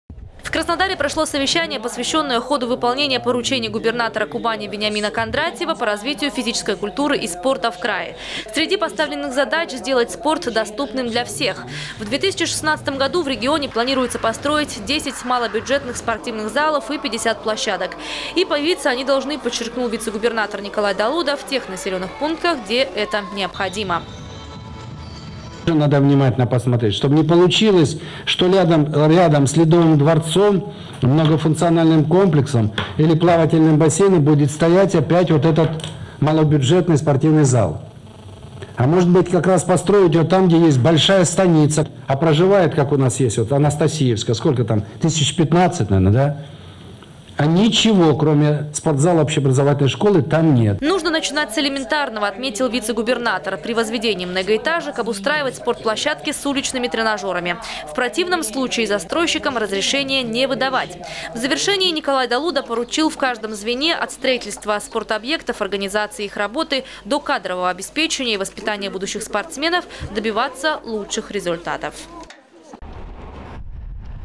Кубань - лидер России по развитию массового спорта. Новости Сочи Эфкате
Такого статуса край достиг впервые. Однако в микрорайонах крупных городов, во многих станицах и хуторах до сих пор не хватает спортивных залов и площадок.
Сочи. Новости.
Телеканал Эфкате.